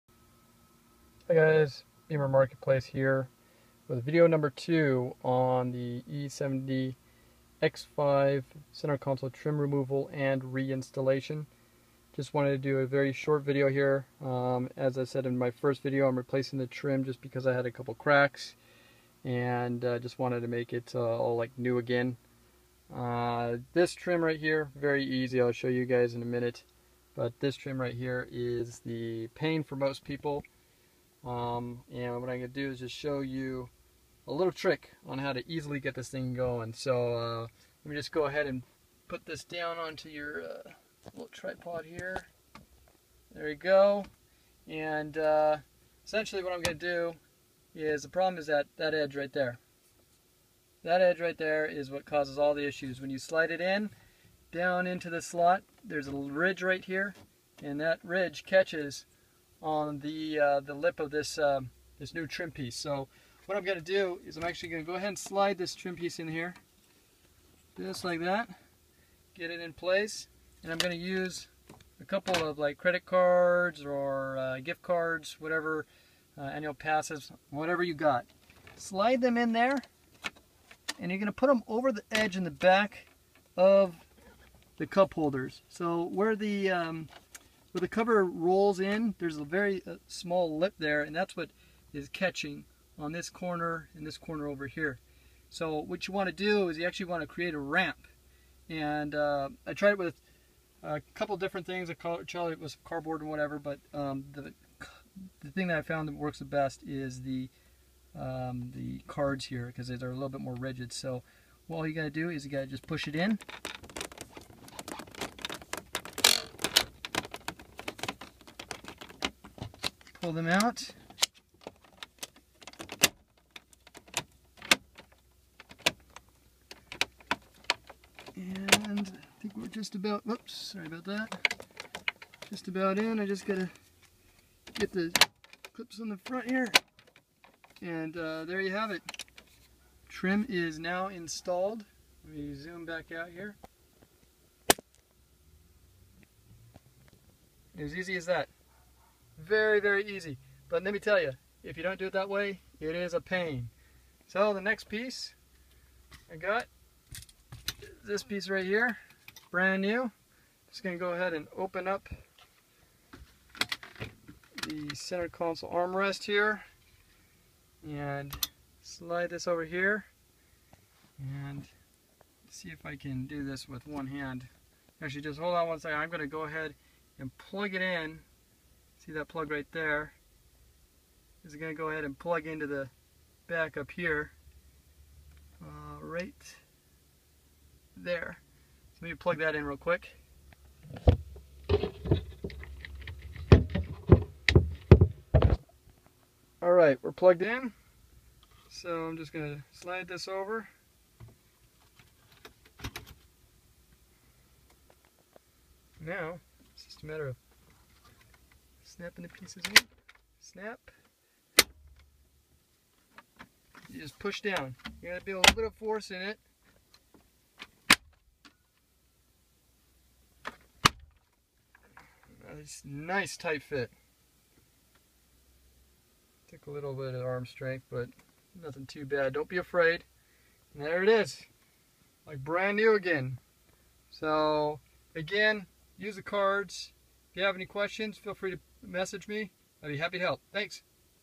BMW E70 X5 - Center Console Trim Removal & Installation (2 of 2)
This is a short video showing how to easily install both trim pieces of the E70 X5 center console. The cup holder surrounding trim is by far the most difficult piece to install without the proper technique. I'll explain the issue many run into along with the technique I use to quickly install this trim piece with no special tools.